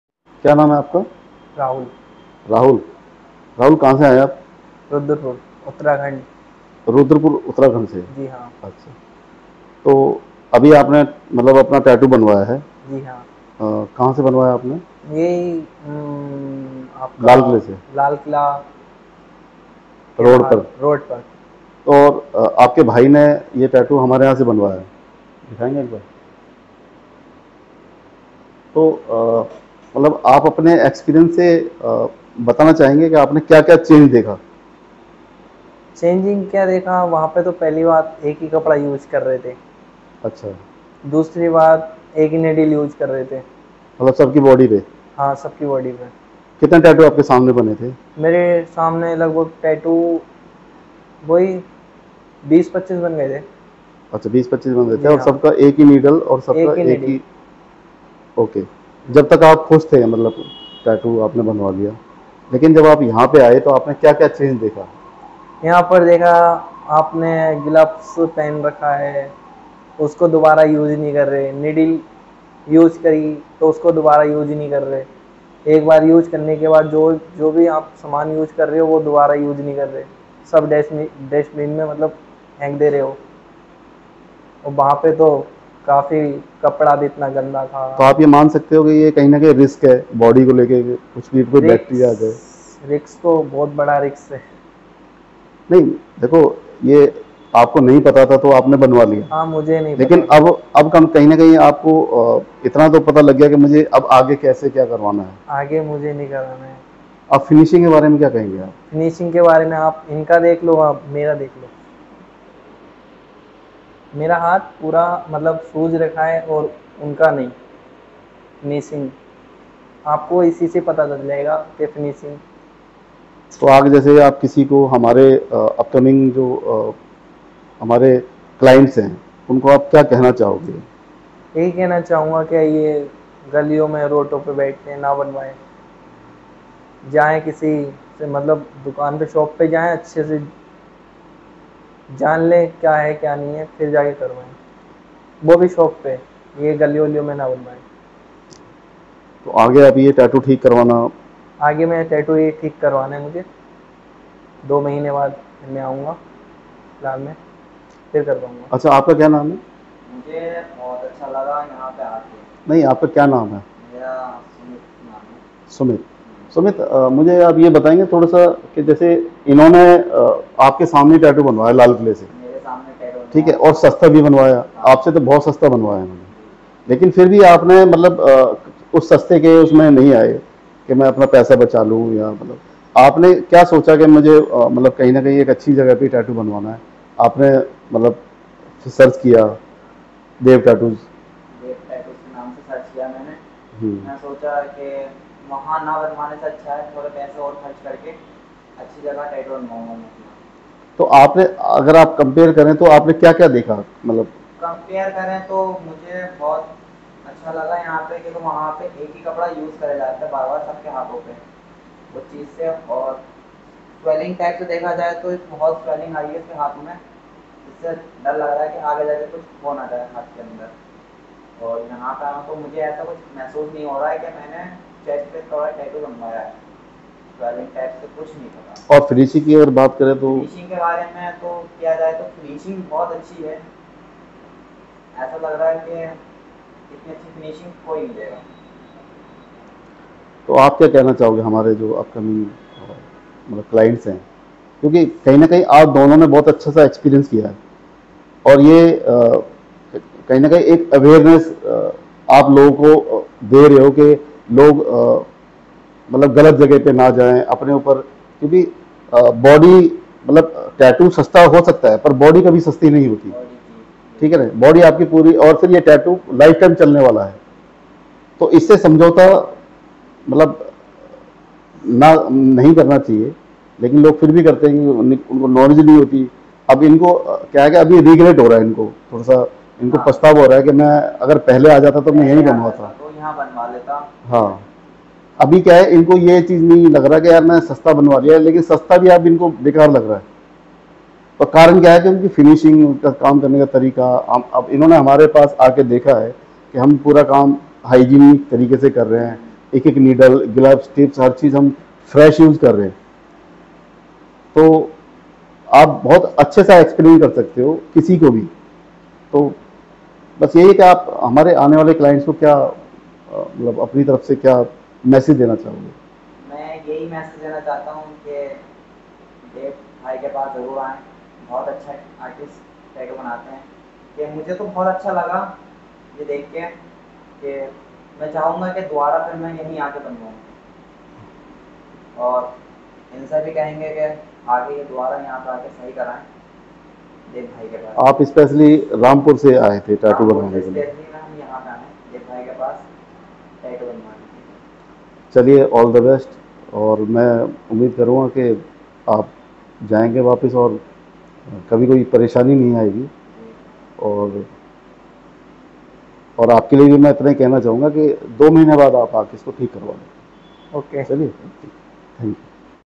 Original Tattoo Feedback By 2 Clients With Roadside & Tattoo Studio Experience | Good vs Best Tattoo
Dev Tattoos Tattoo Artist in Delhi